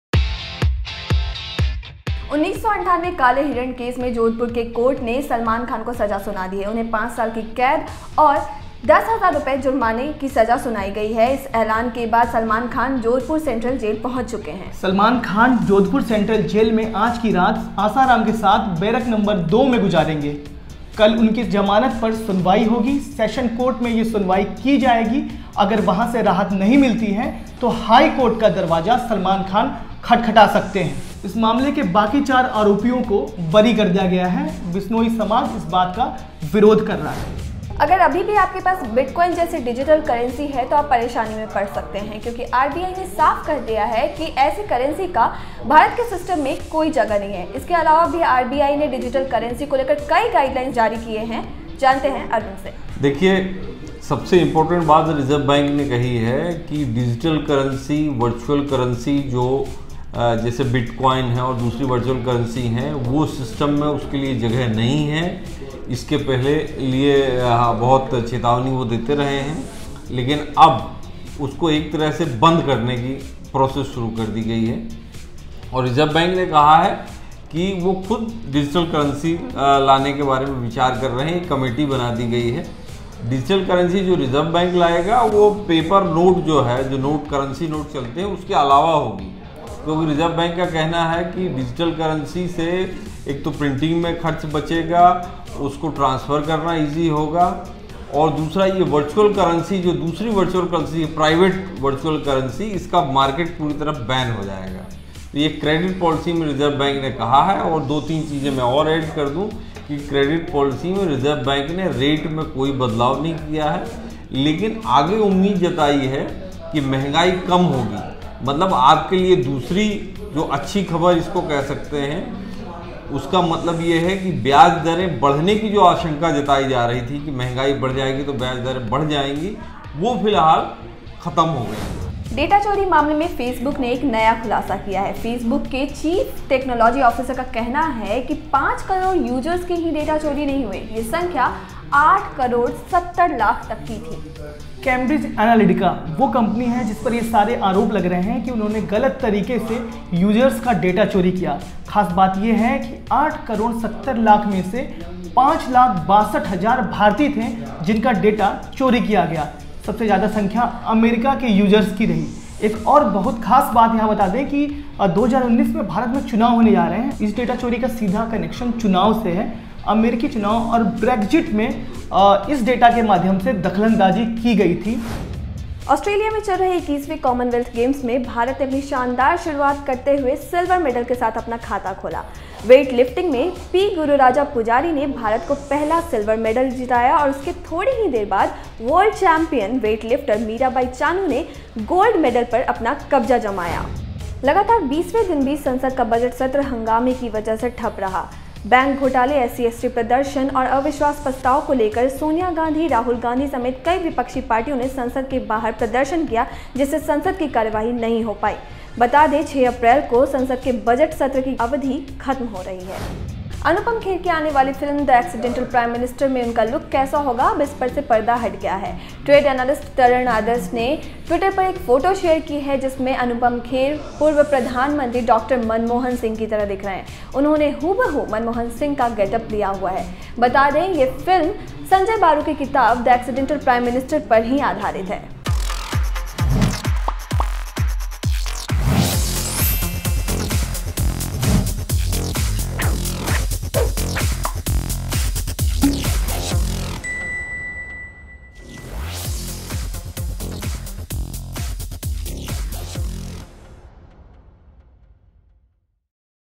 QExpress: Salman Khan की ‘जेलयात्रा’, RBI ला सकता है डिजिटल करेंसी | Quint Hindi
काले हिरण के शिकार से लेकर सलमान की ‘जेलयात्रा’ तक,हर सवाल का जवाब
रिजर्व बैंक ला सकता है डिजिटल करेंसी, दूसरी वर्चुअल करेंसी बैन
फेसबुक पर 8.7 करोड़ यूजर्स का निजी डेटा लीक, भारतीय भी शामिल
CWG 2018: भारत की शानदार शुरुआत, चानू को मिला गोल्ड मेडल
मनमोहन सिंह वाले अंदाज में दिख रहे हैं अनुपम खेर